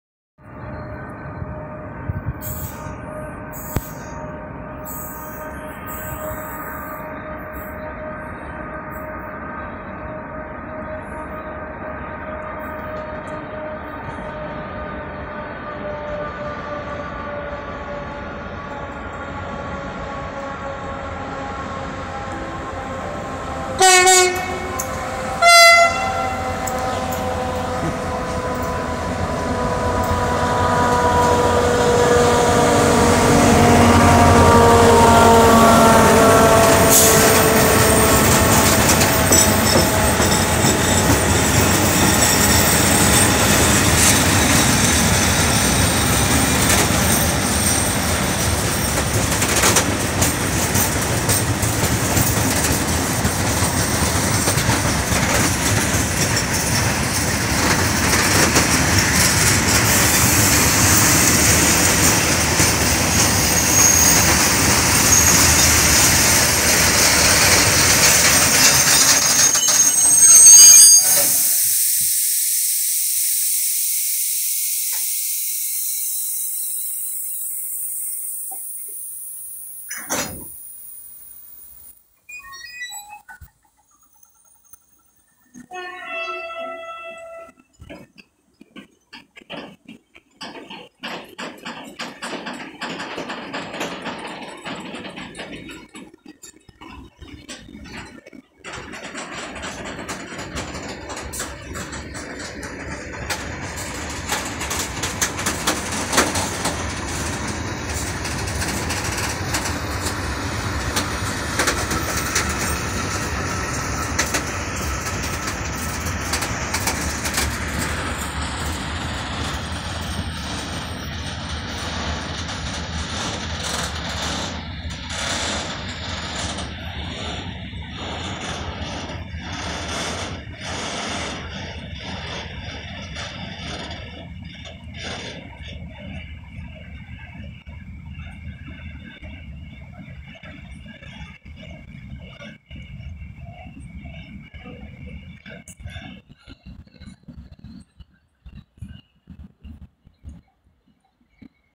Locomotora 335-010 "Transfesa" con el chatarrero de Vallecas en Vicalvaro. 14/12/2023.
Transfesa rented,  Euro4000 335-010 ,Alpha Trains locomotive hauls loaded scrap train coming from Vallecas. It stops and goes at Vicalvaro station. 14/12/2023.